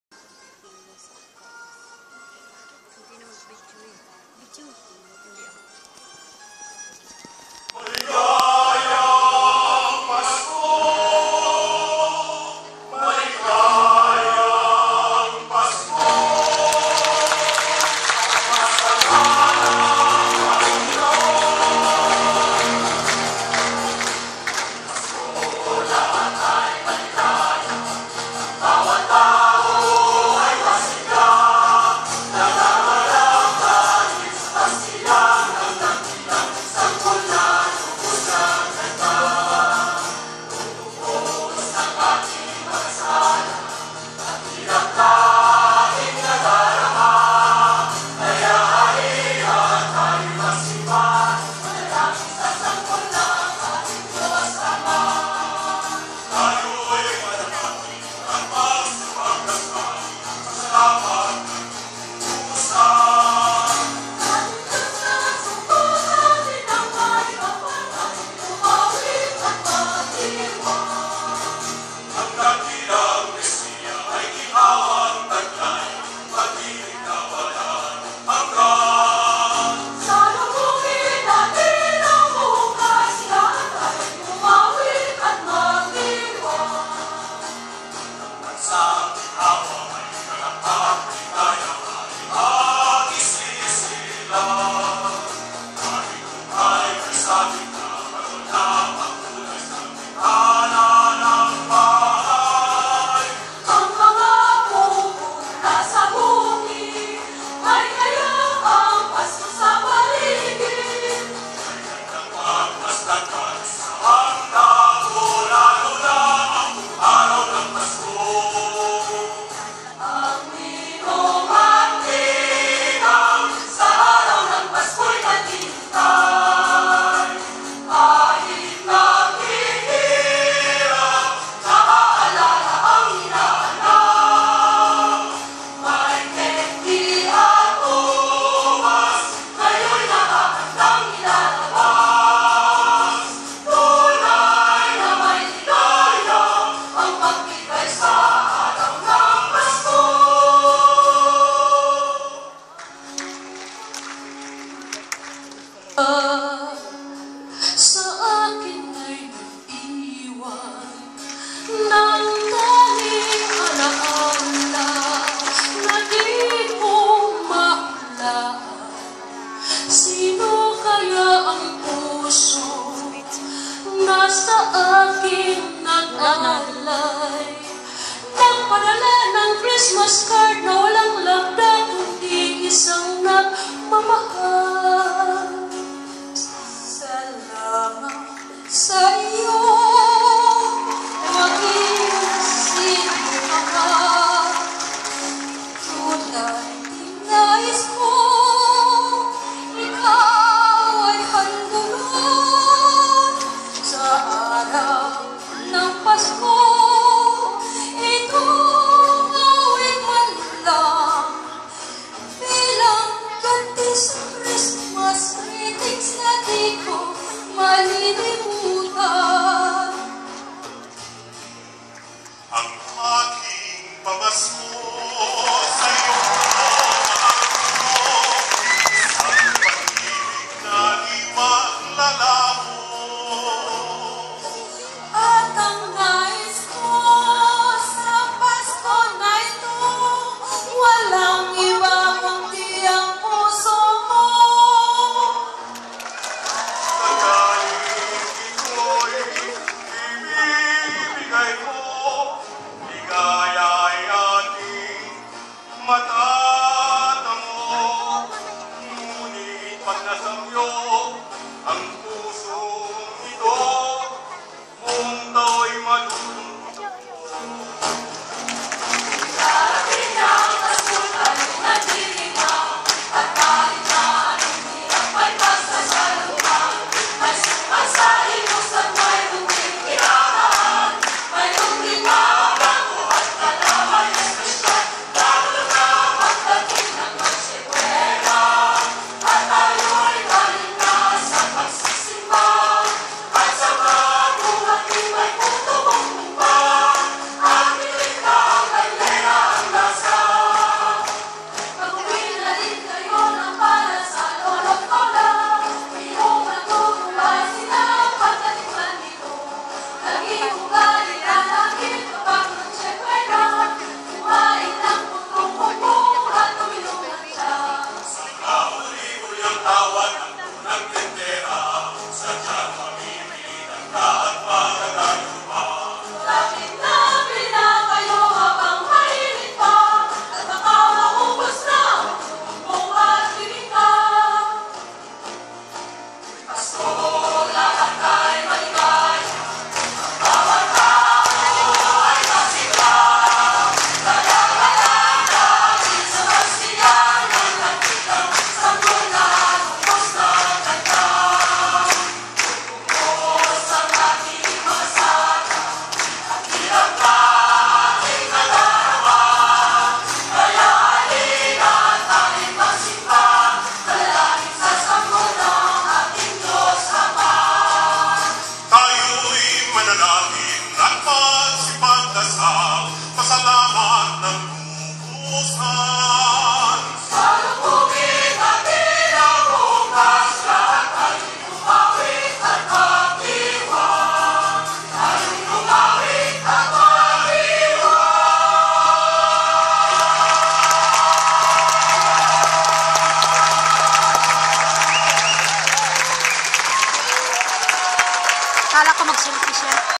PHILIPPINE COMMUNITY CHOIR in LEBANON
The Embassy of the Philippine in Lebanon celebrating the first Christmas festival in Lebanon 2014